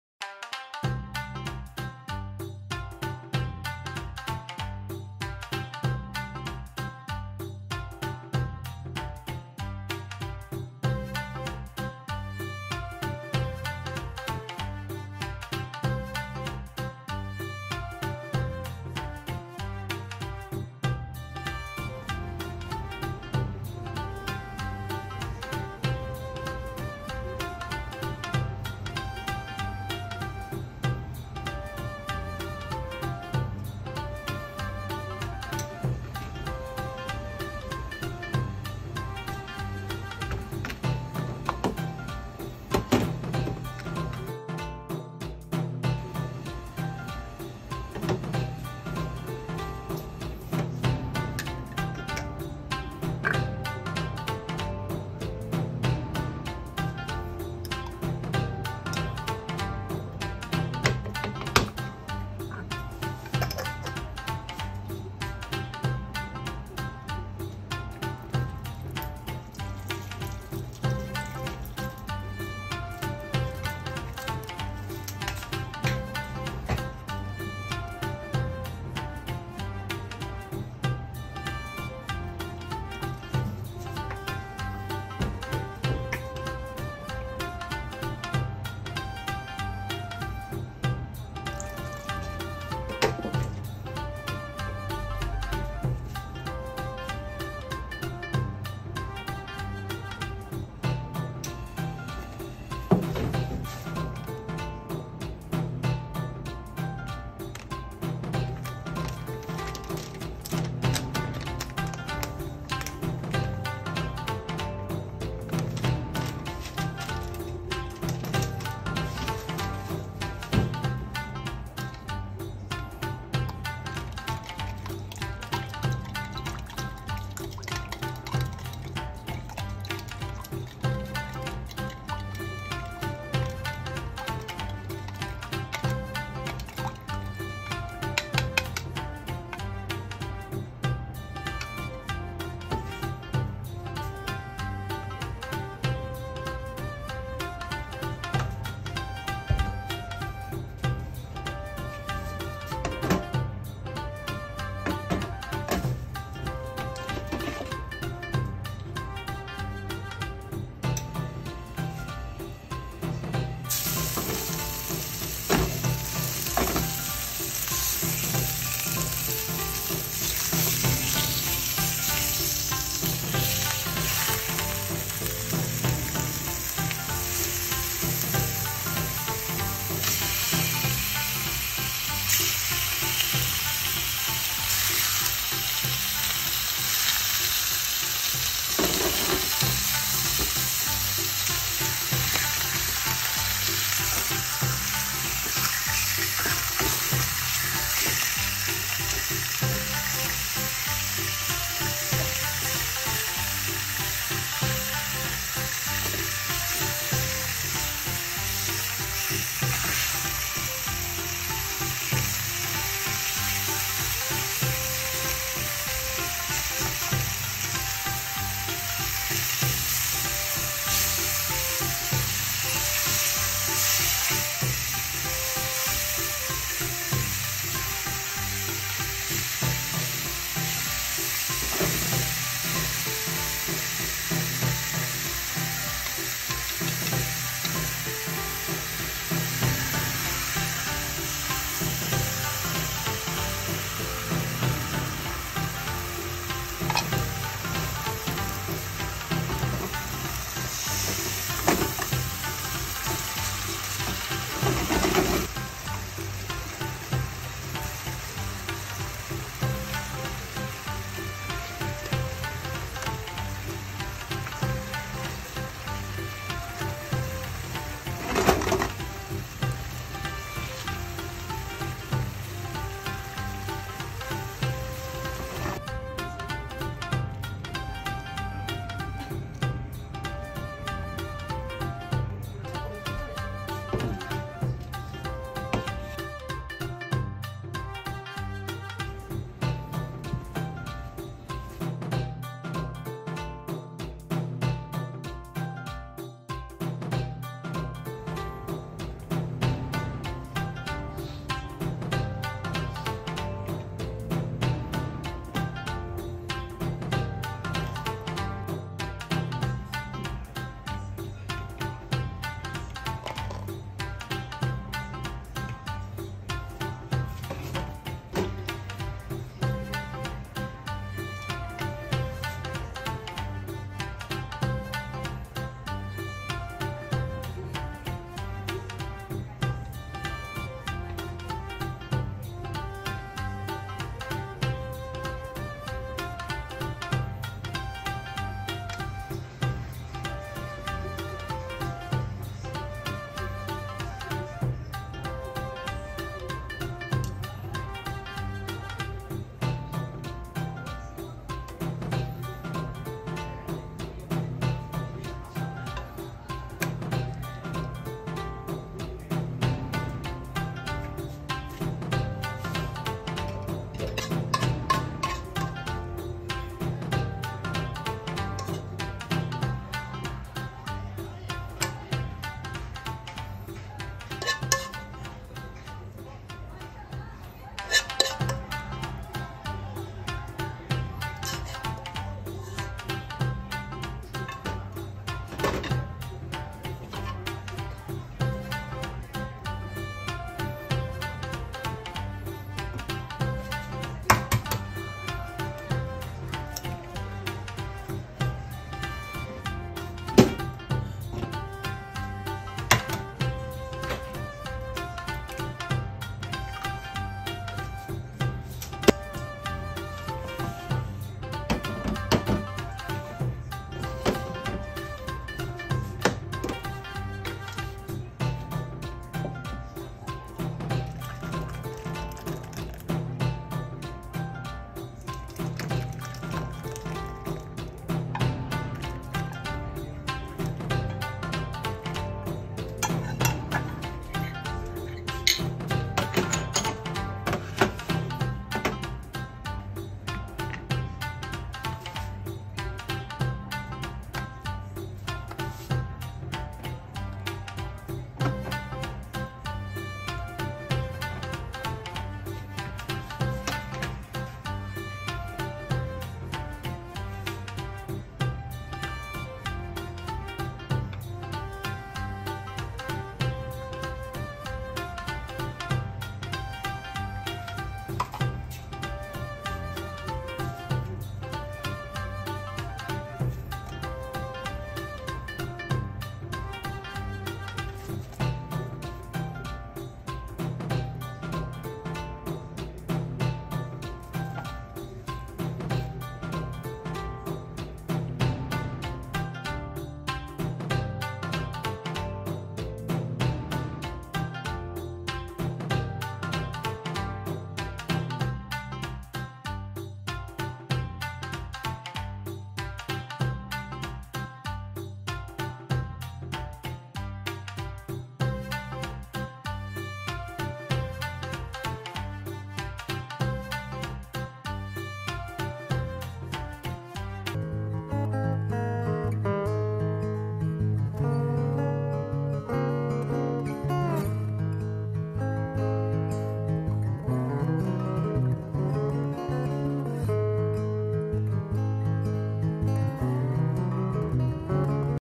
【garlic pork rice bowl 】Japanese breakfast preparation
This is a video of Japanese breakfast being prepared.For today's breakfast, I will make
garlic pork rice bowl.